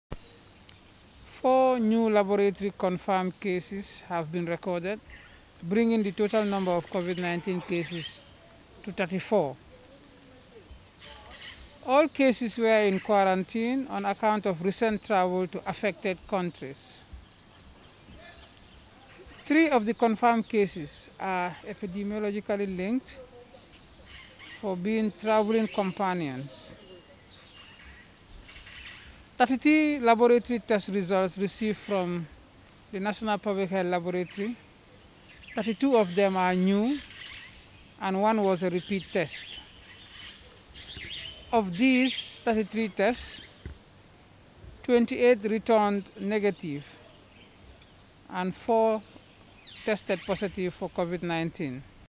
EnglishNews It’s the 78th national situation report since the confirmation of the first case of the
It’s the 78th national situation report since the confirmation of the first case of the novel corona virus in the Gambia on the 17th march 2020. 4 new laboratory confirmed case have been recorded bringing the total number of COVID 19 cases to 34